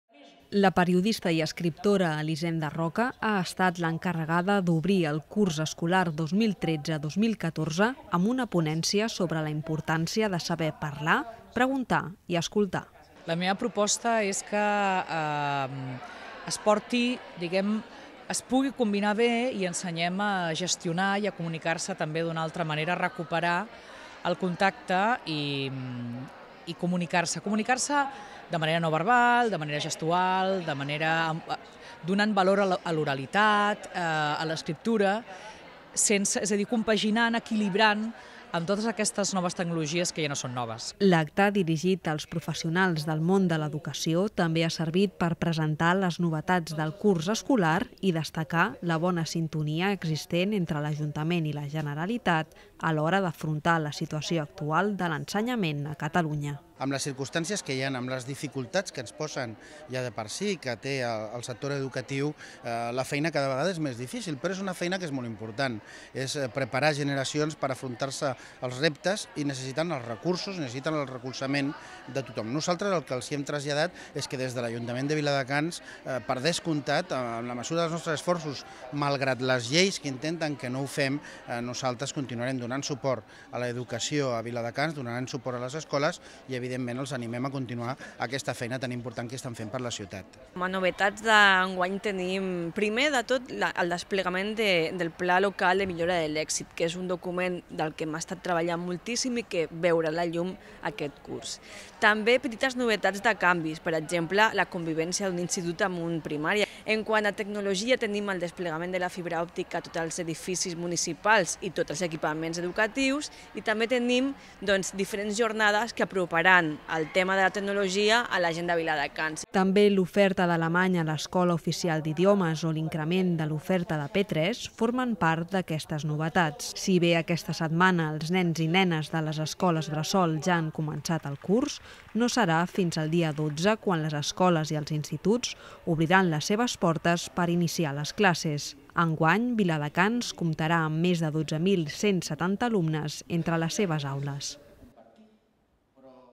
La periodista i escriptora Elisenda Roca ha inaugurat el curs escolar 2013-2014
La periodista i escriptora Elisenda Roca ha estat l’encarregada d’obrir el curs escolar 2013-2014 amb una ponència sobre la importància de saber parlar, preguntar i escoltar.